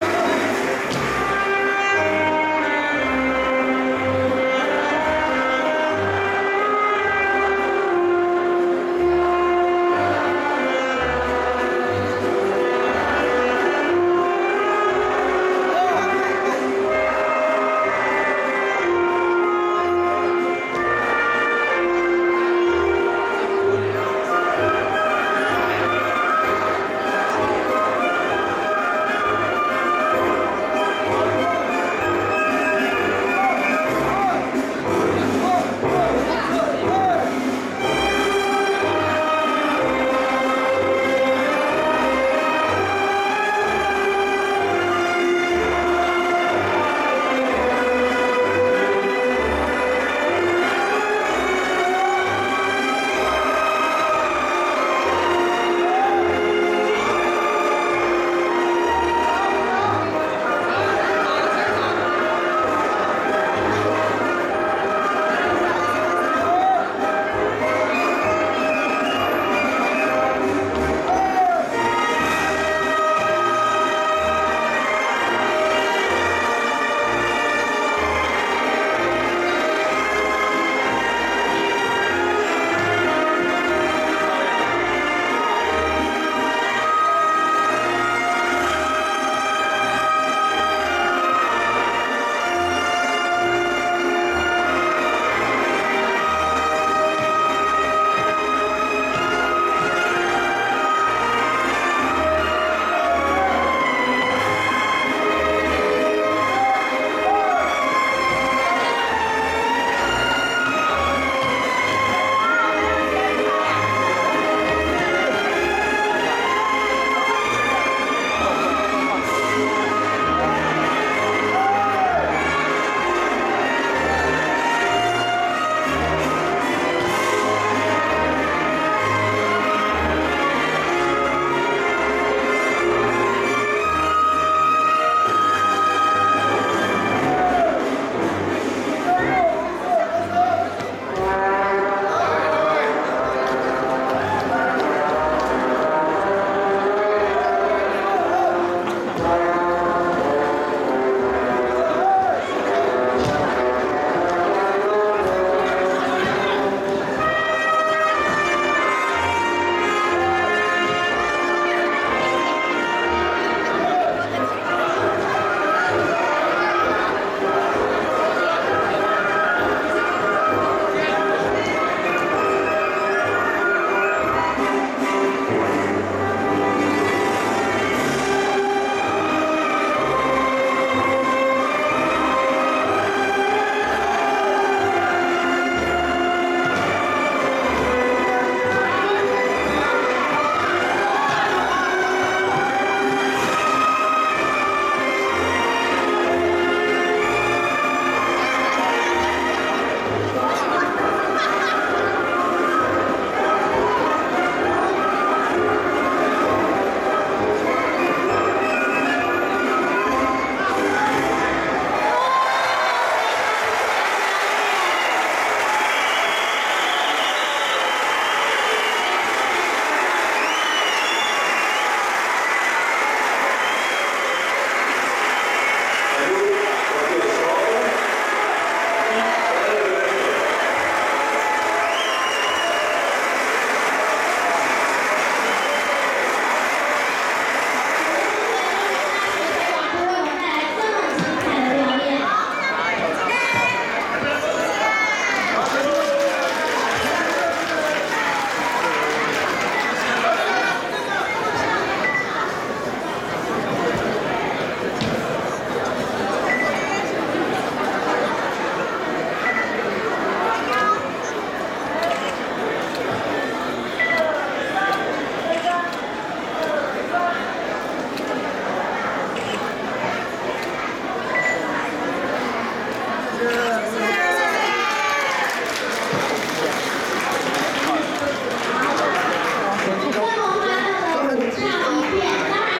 2017龍潭迎新聯歡大舞會 蔡輝煌老師 86
2017龍潭迎新聯歡大舞會-蔡輝煌老師
時間:2017/01/01 周日 11:30-18:00
地點:龍潭生生圓大舞台
桃園市龍潭區大昌路一段169號
記得的是歡樂舞步
忘記的是氣質昇華